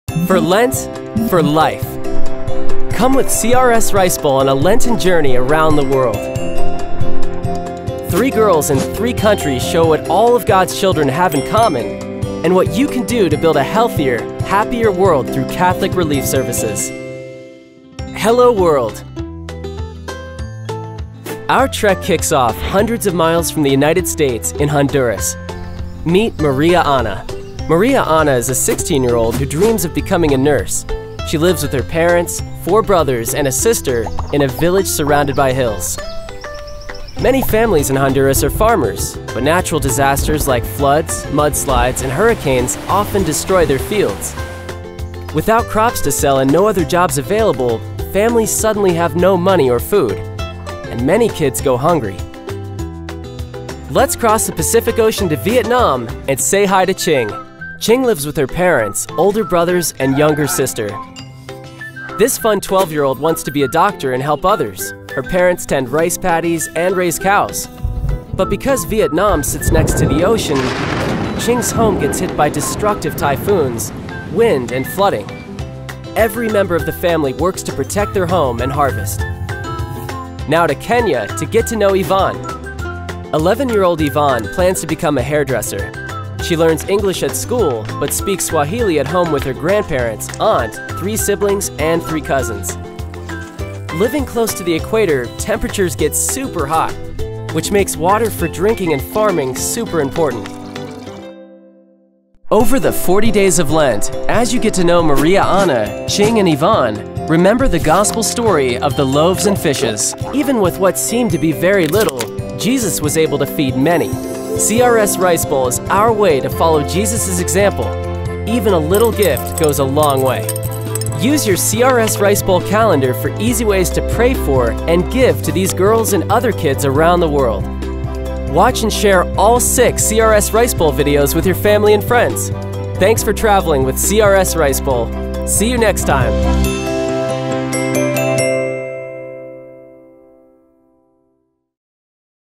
Hello, World! | CRS Rice Bowl 2020
Travel with CRS Rice Bowl on a Lenten journey around the world.  Three girls in three countries show us what all of God’s children have in common and what you can do to build a healthier, happier world with Catholic Relief Services. The first in a series of six Rice Bowl 2020 videos.